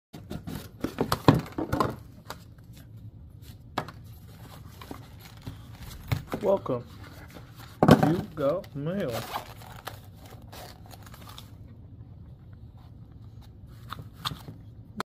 Unboxing mantis x10
no coaches in canada, so decide to invest in a virtual  coach